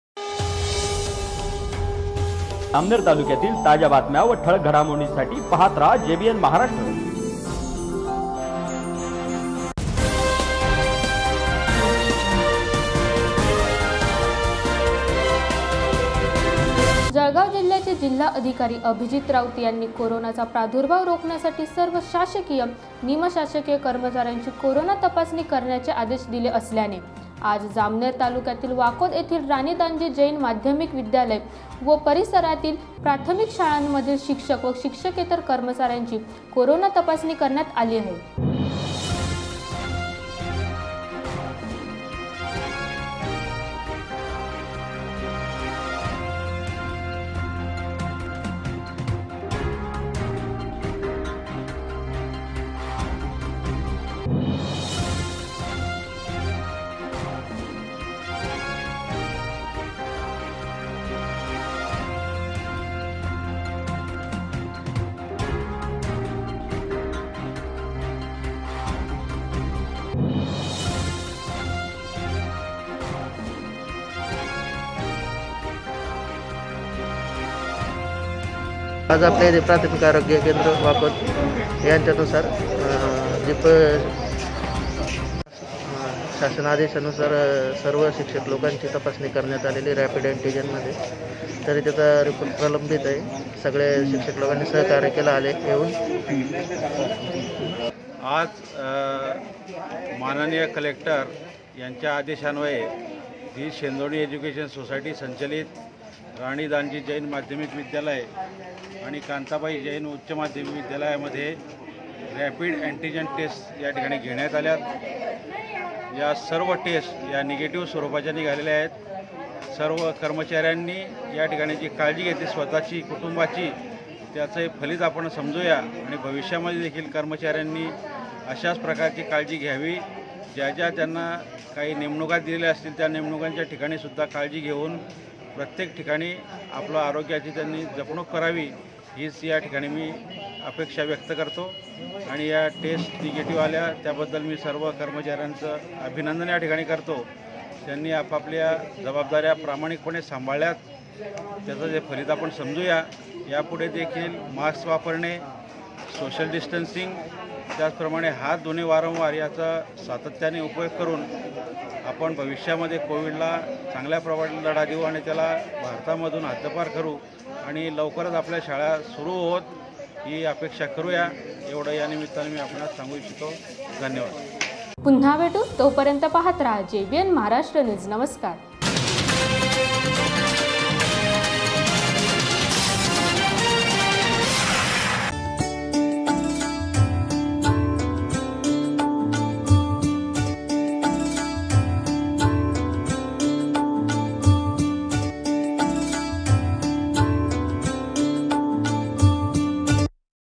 jbn maharashtra news jalgaon
कोरोनाचा प्रादुर्भाव रोखण्यासाठी सर्व शासकीय निमशासकीय कर्मचाऱ्यांचे  तपासणी करण्याचे आदेश जिल्हाधिकारी अभिजित राऊत यांनी दिली